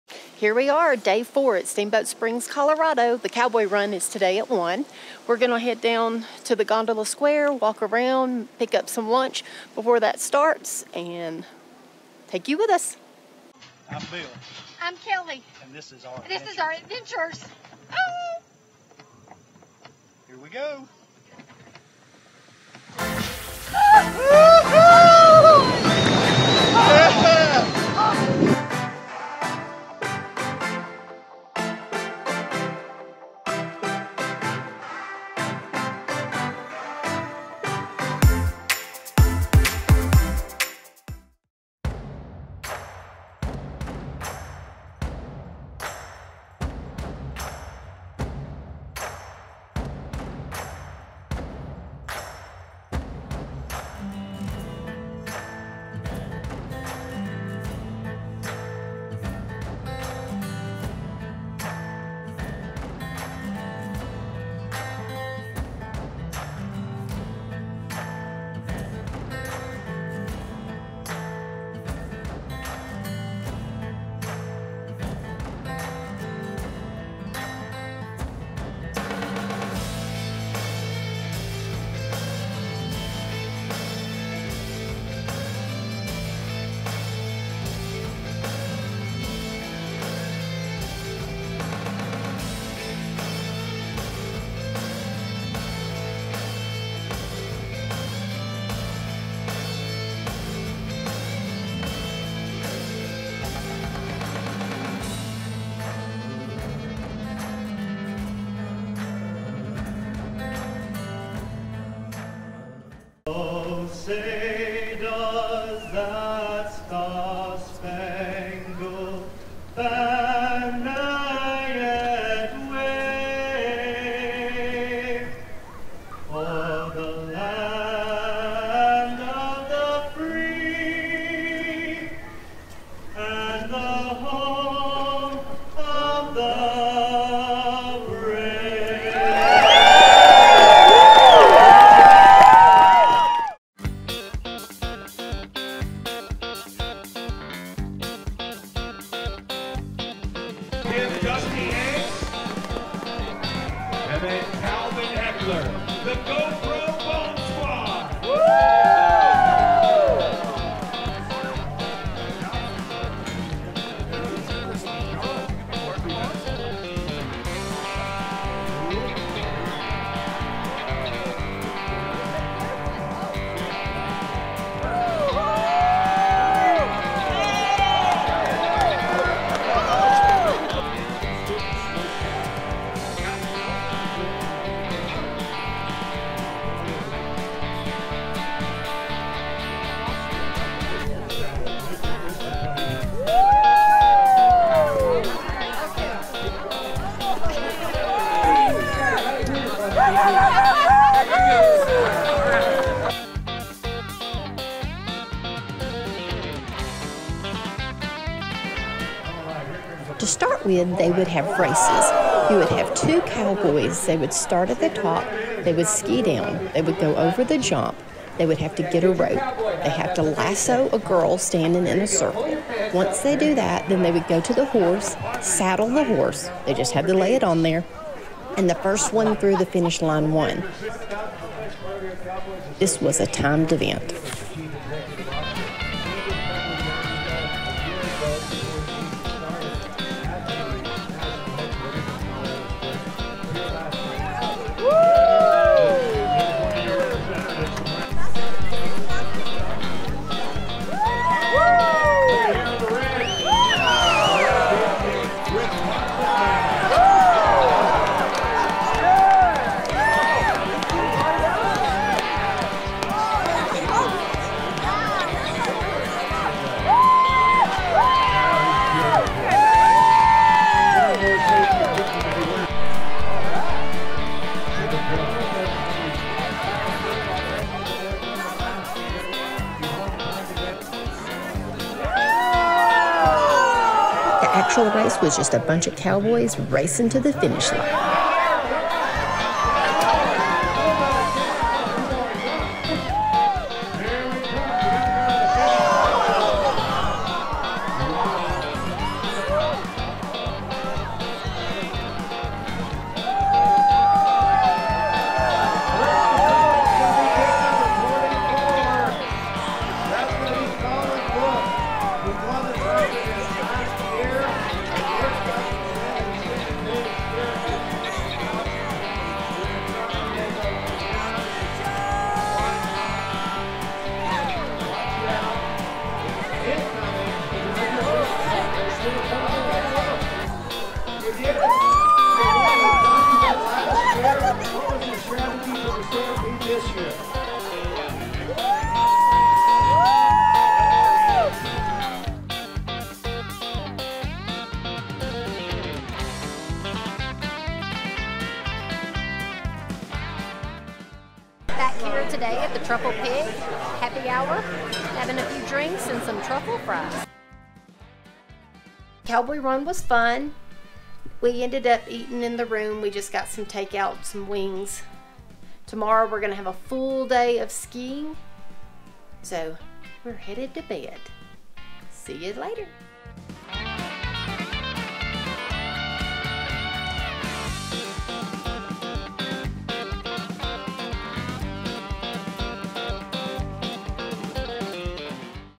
January 21 Steamboat Spring Cowboy Run
We loved the cowboy run.
It was great we had great seats for us but not for videoing. I did use some of Steamboats video to get some close-up action!!!